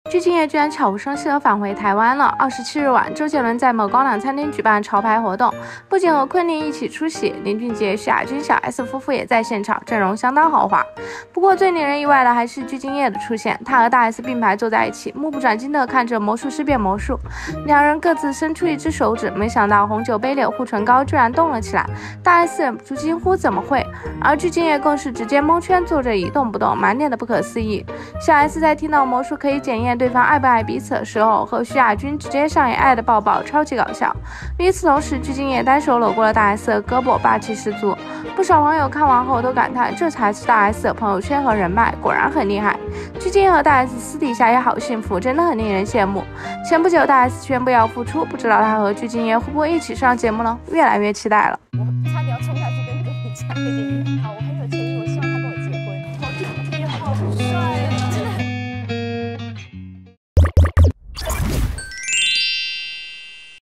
具俊瞱爲愛二度來台！和大S出席潮牌活動見周傑倫夫婦，單手摟妻子胳膊看魔術霸氣十足#大s ，具俊曄
具俊瞱和大S出席周傑倫的潮牌店活動，現場大咖雲集。

文案：于謙首席燙發師

後期：阿偉